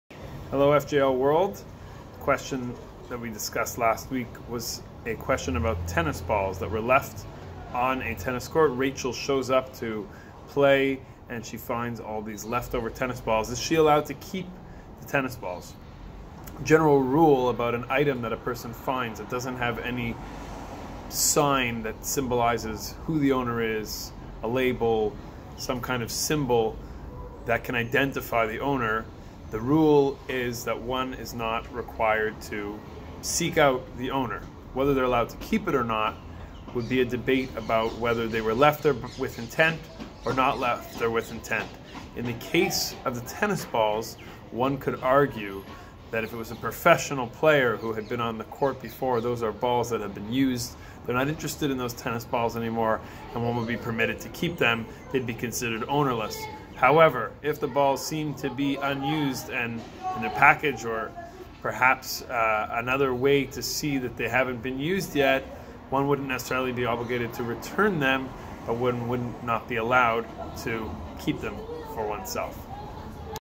Can you take tennis balls left from a previous game?
Rachel came to the tennis court for her morning warm-up and found tennis balls left behind from someone who had been playing there before. Would Rachel be permitted to take the balls?

(Question adapted from JLIC Canada)